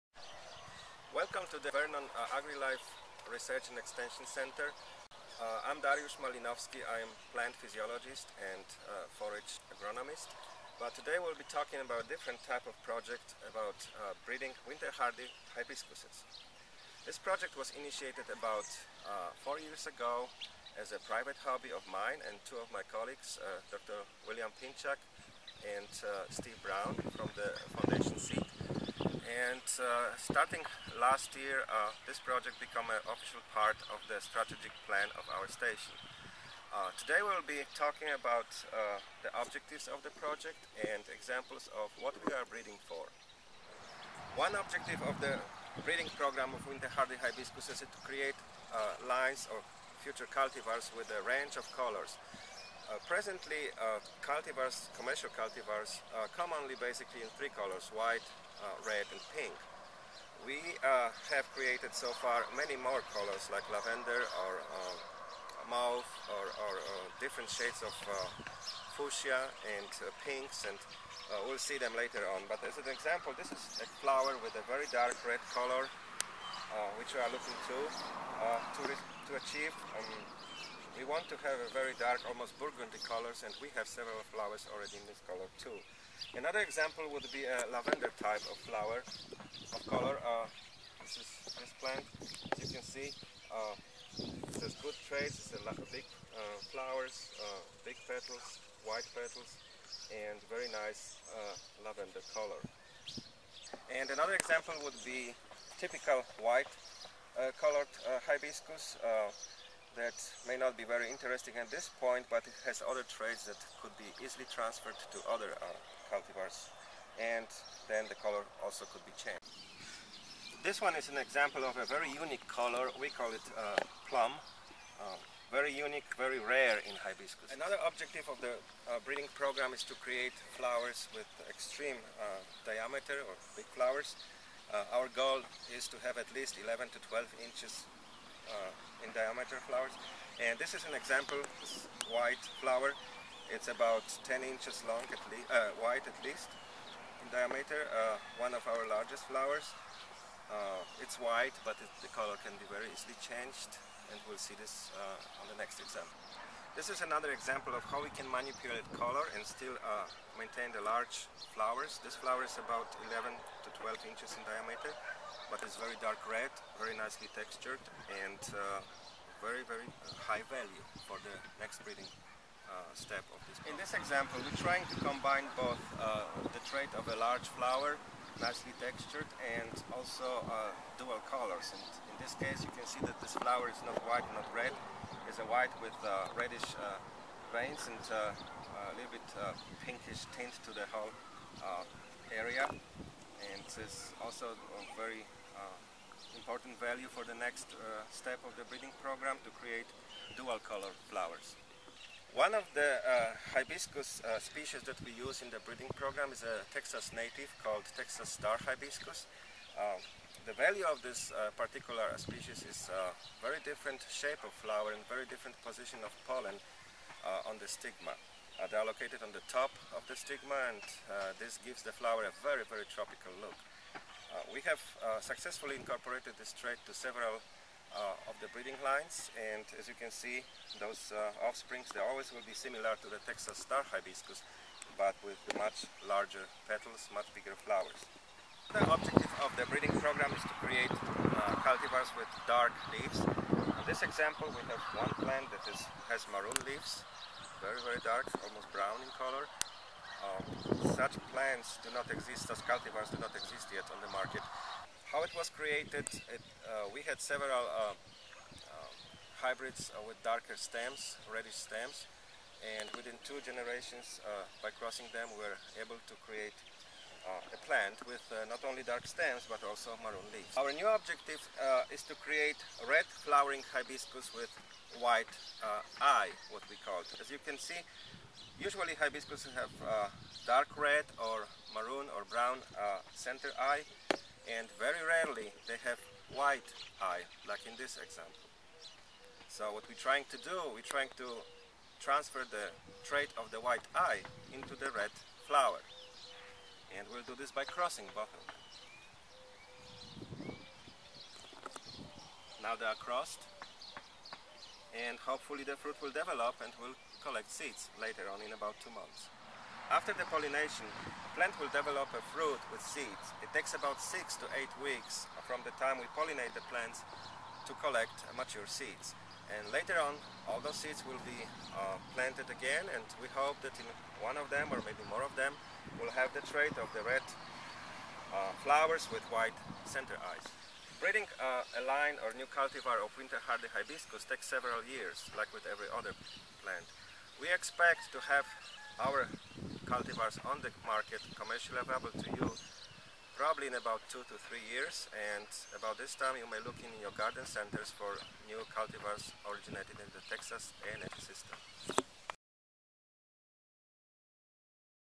AgriLife Hibiscus Breeding.wmv
Dr. Dariusz Malinowski with Texas AgriLife Research discusses breeding new hibiscus flowers in larger sizes and more vibrant colors.